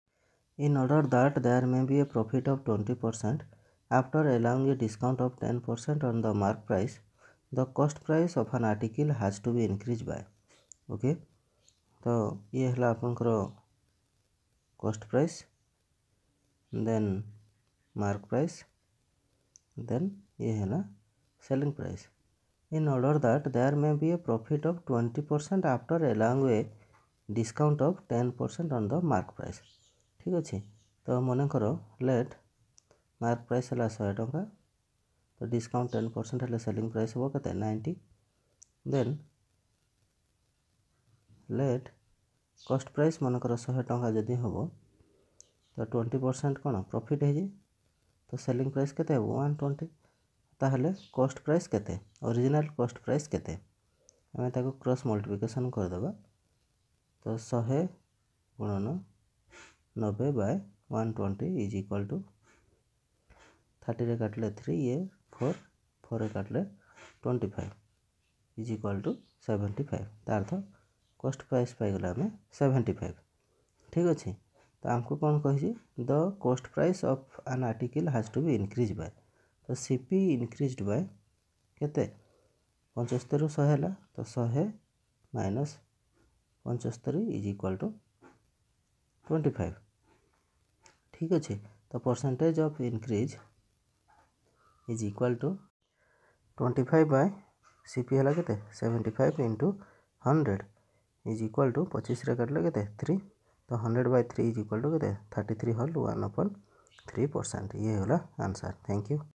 In order that there may be a profit of 20% after allowing a discount of 10% on the marked price
In order that there may be a profit of 20% after allowing a discount of 10% on the marked price, the cost price of an article has to be increased by
(A) 30%
(B) 33%